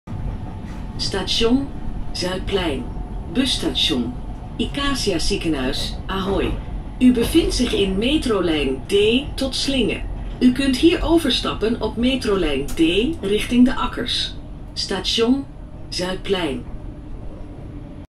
Rotterdam (RET Metro); announcement for next stop; Zuidplein (20110707) (1)
Automatische halteafroep opgenomen in de Rotterdamse metro (RET).
Betreft afroep voor station Zuidplein + melding busstation - Ikazia ziekenhuis & Ahoy + melding eindbestemming (Slinge) & overstapmogelijkheden.